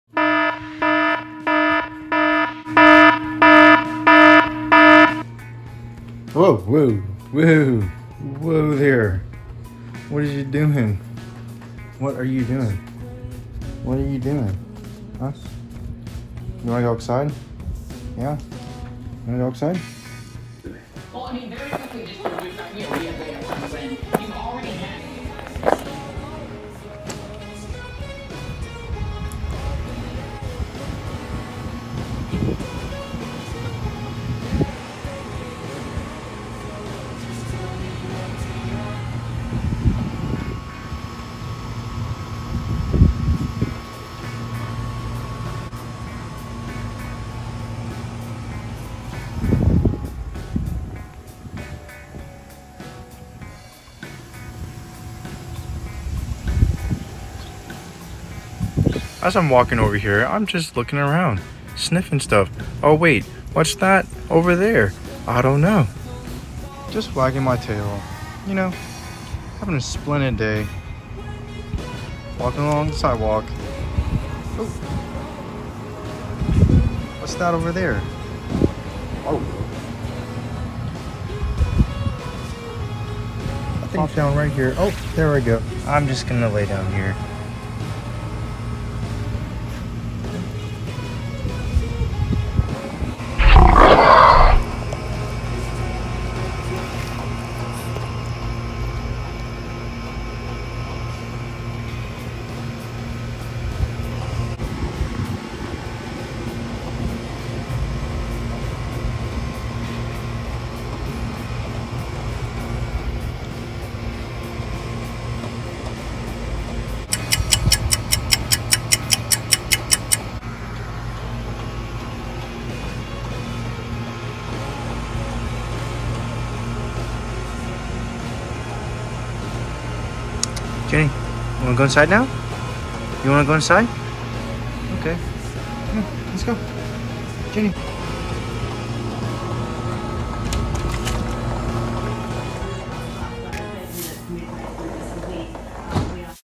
Life of Jenny. first short film
filmed for a class assignment. hope you guys enjoy it. my first edited film.